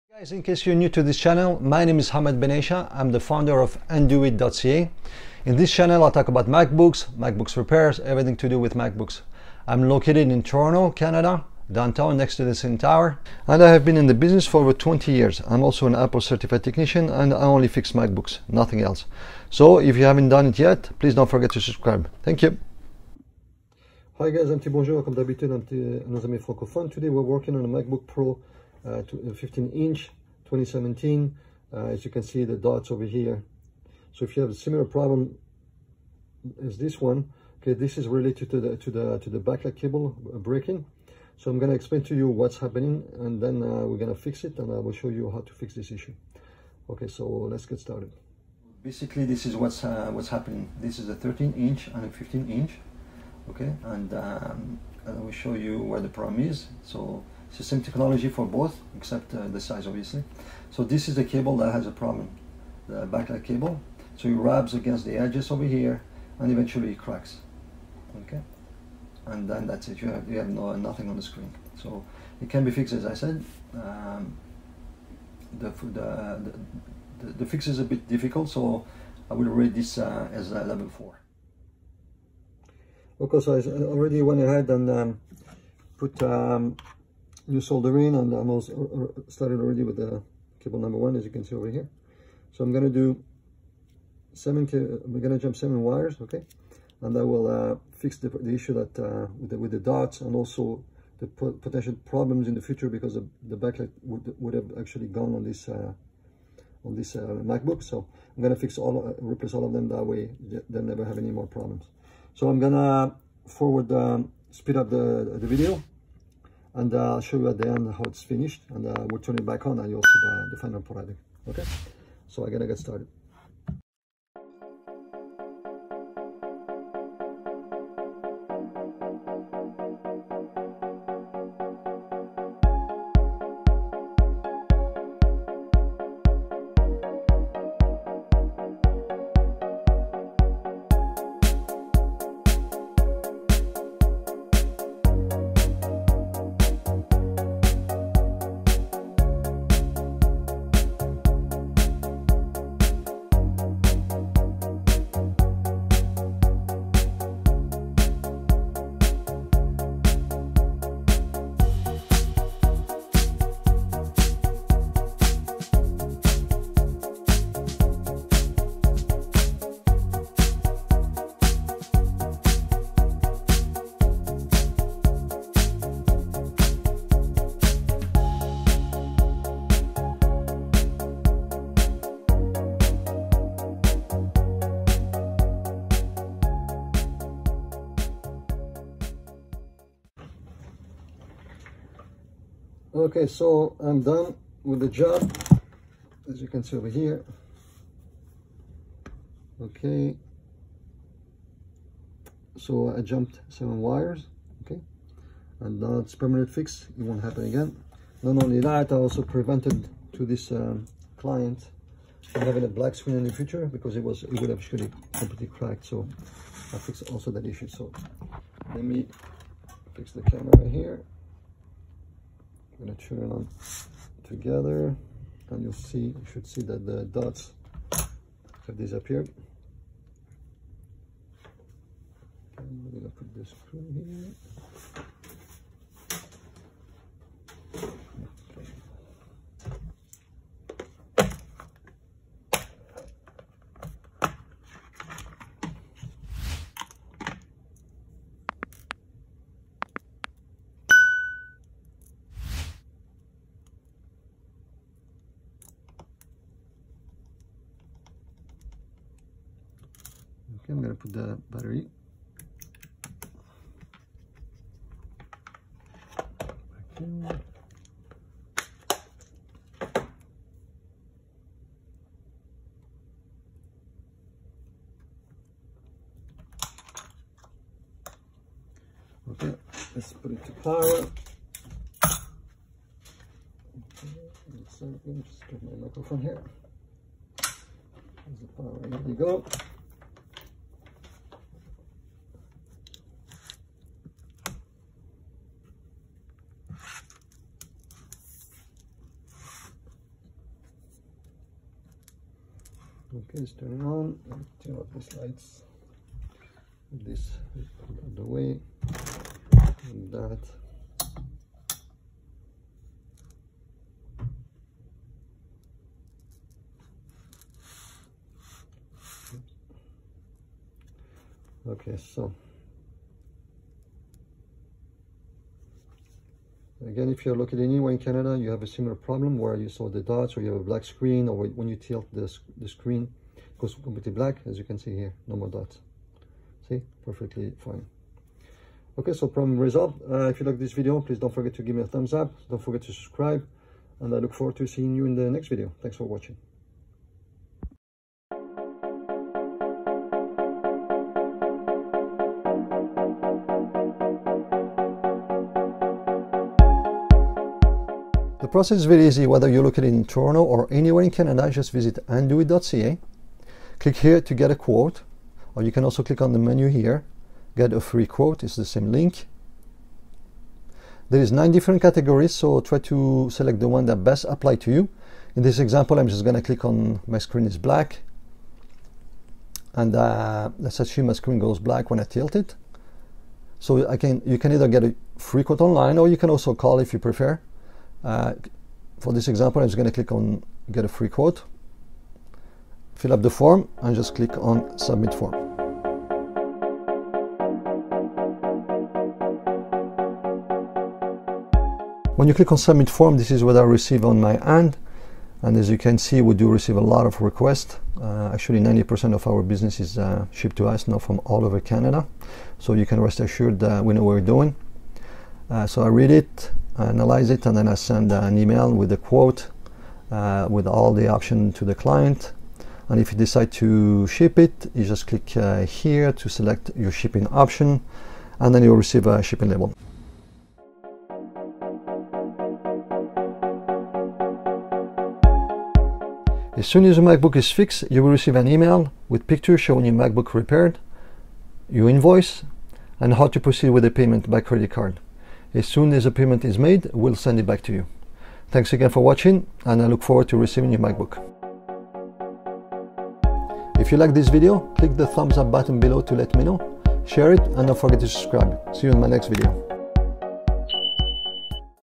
🇨🇦 MACBOOK LCD WITH SPOTLIGHTS AT THE BOTTOM OF THE SCREEN (DOTS) RESULTING IN BLACK SCREEN, FIXED!
🇨🇦If you have a MacBook screen similar to this video, or a screen that goes completely black when it is moved, it is an issue with the backlight cable, if you need help fixing it make sure to check out the end of the video.